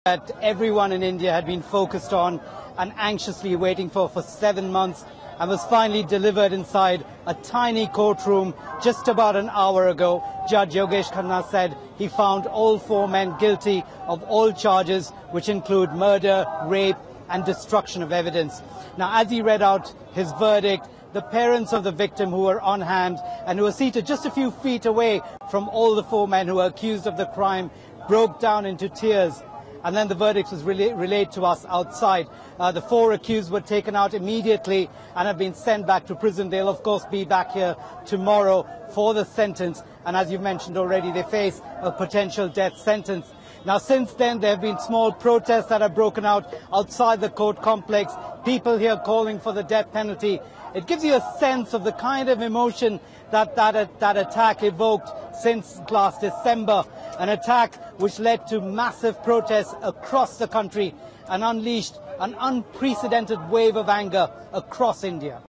BBC News Delhi gang rape trial Four convicted for fatal attack
Four men have been found guilty of the fatal gang rape of a medical student in the Indian capital Delhi last December.

The 23-year-old woman was brutally assaulted on a bus and died two weeks later, which led to days of violent protests across India in a wave of unprecedented anger.

Sanjoy Majumder reports from Delhi.